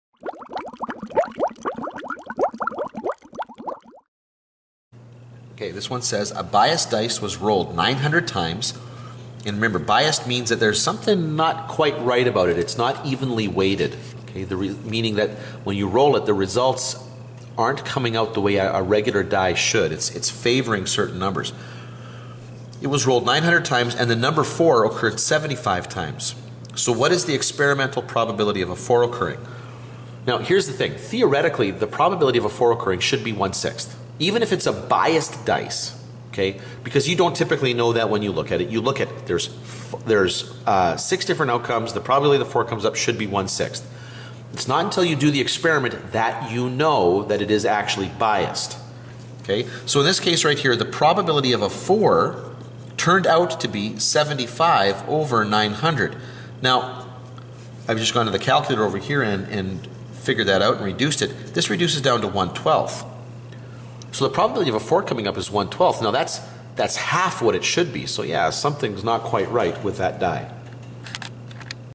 Math 30-2 Probability Review #3
This video is about experimental probability